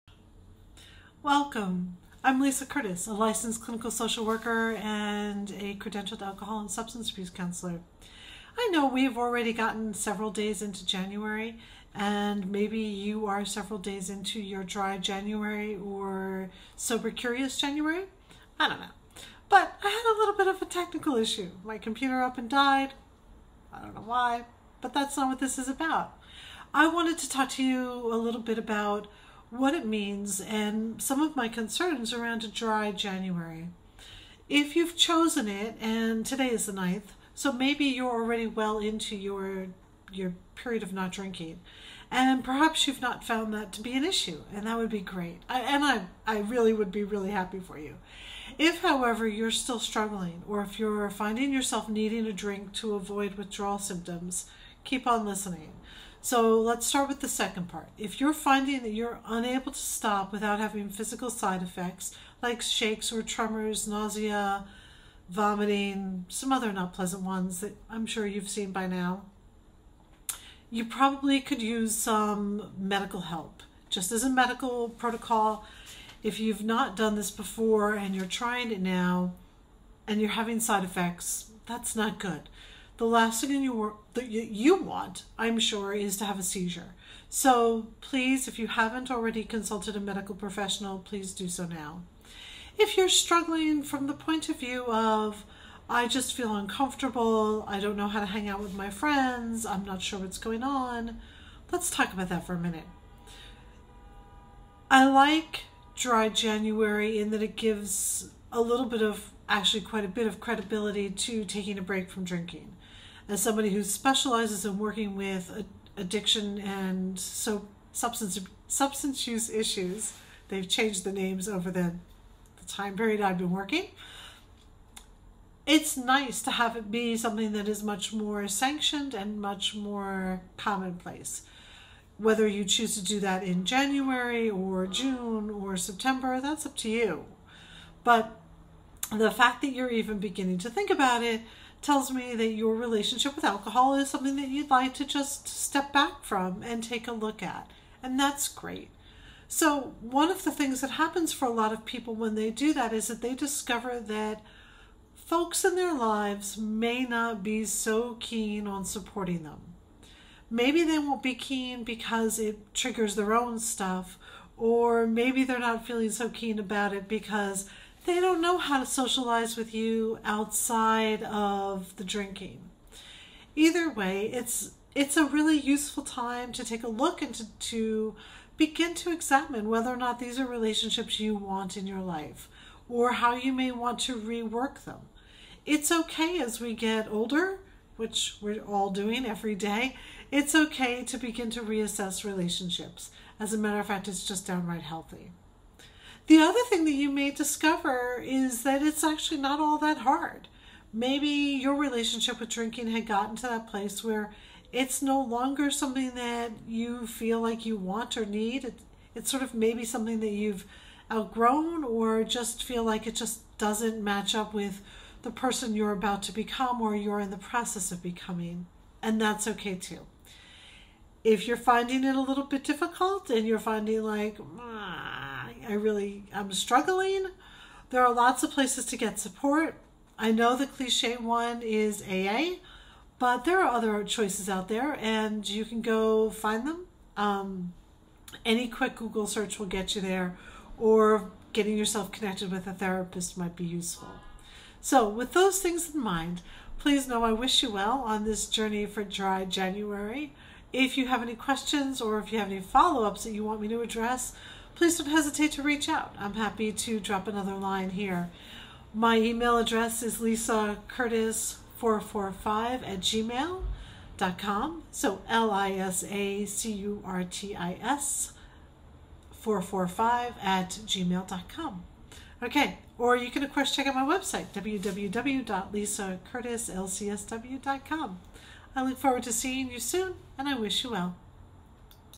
Dry January 2023
Some thoughts to help you navigate a Dry January and a reminder for when it's time to seek medical support.